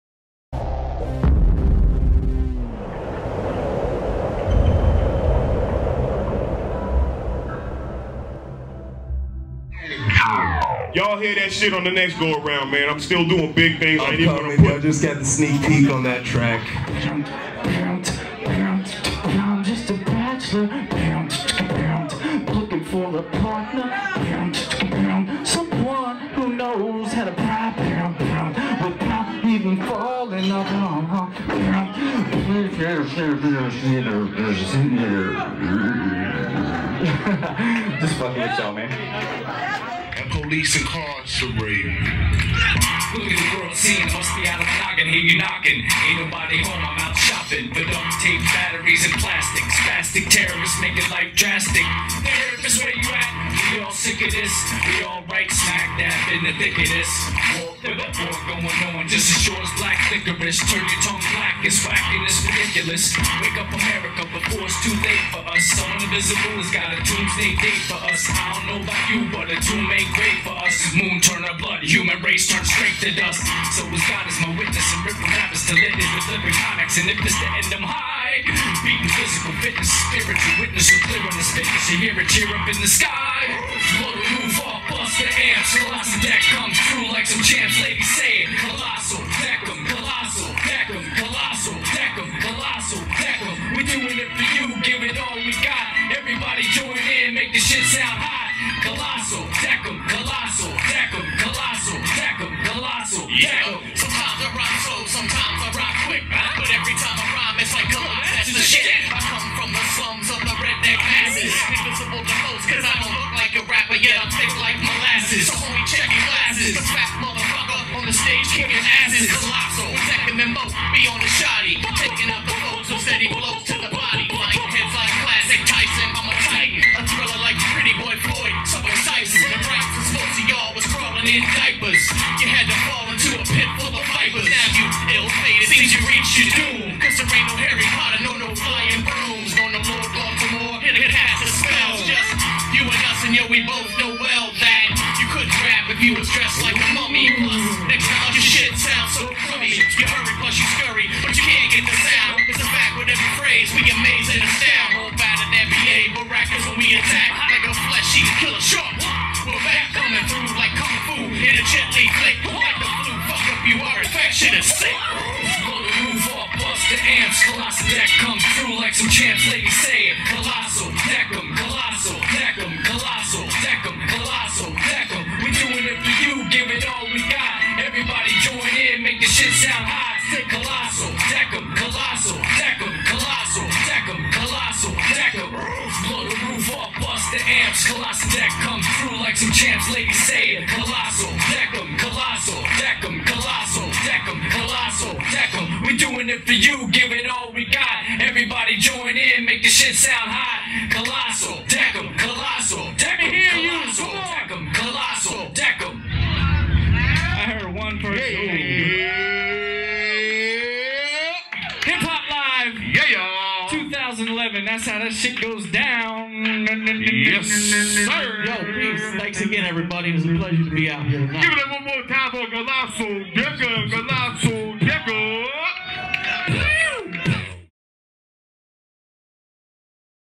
Kolassal & Decm
Surf Skate Music Culture is local music's number on fan! Take a look at Kolassal & Decm doing their thing at the Island Bar. Guys, if you haven't been able to see these groups live yet now is the time to get off your fat ass and "Occupy a frickin hip-hop show!"